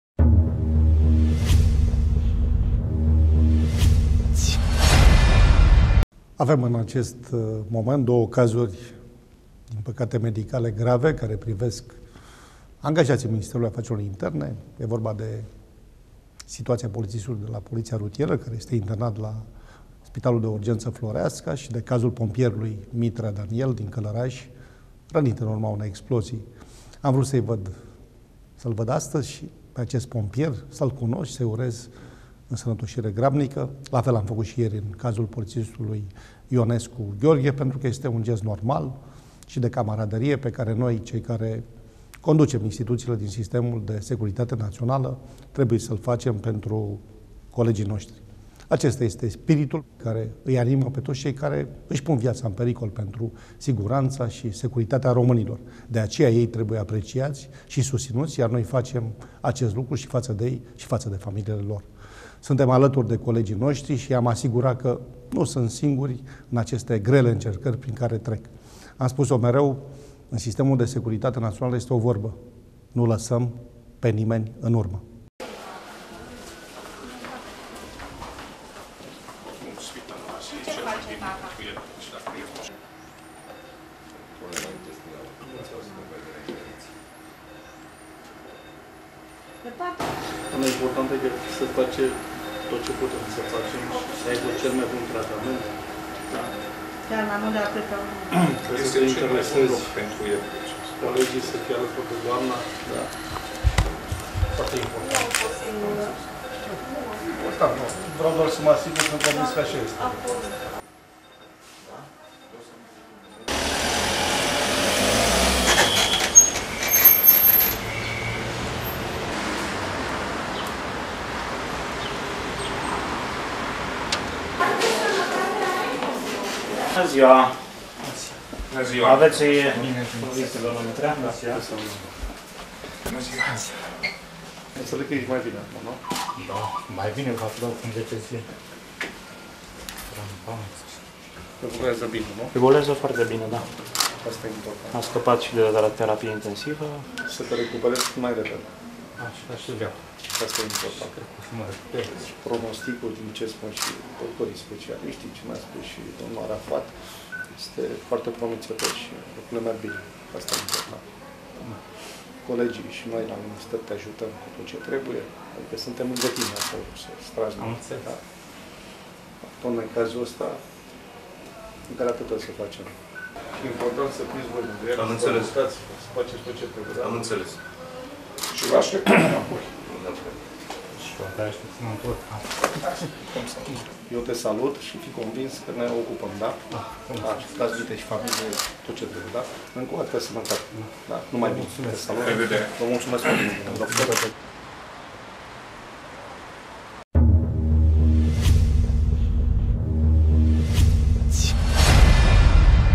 Vicepremierul Gabriel Oprea a vizitat, la spital, un polițist și un pompier răniți in misiune
Ministrul Afacerilor Interne, Gabriel Oprea, l-a vizitat marți, la Spitalul Clinic de Urgență, Chirurgie Plastică, Reparatorie și Arsuri, pe plutonierul adjutant Daniel Mitrea, care a fost rănit în explozia ce a avut loc pe 24 iulie la unitatea de pompieri din Călăraşi